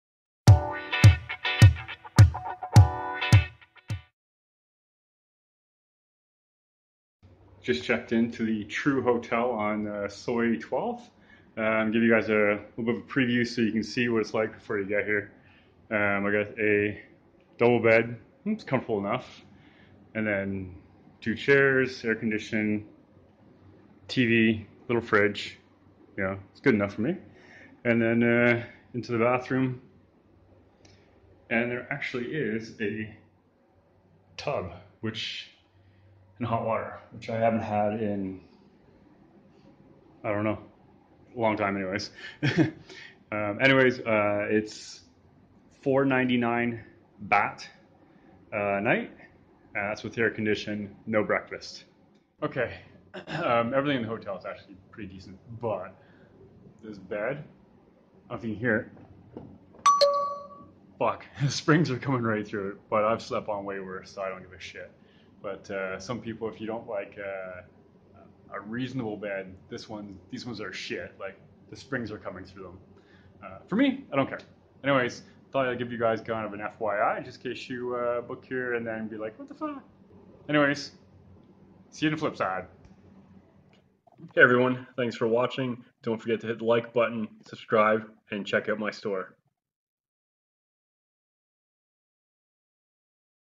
vLog 48 True Hotel Soi 12 Pattaya Jomtien Beach
True Hotel is a small chain in Thailand and this is my first time booking a room with them, through booking.com.
The room has Air Condition, a double bed, and private shower.
Room is decent, but the bed is very uncomfortable, so most might not like it.
499 Baht per night

vLog 48
334/118 Moo 12 Soi Welcome Jomtien beach Rd., Nongprue, Chonburi, 20150 Jomtien Beach, Thailand

Other vLogs from my Travels.

True Hotel Moo 12 Soi Welcome Jomtien beach
Pattaya Pattaya Pattaya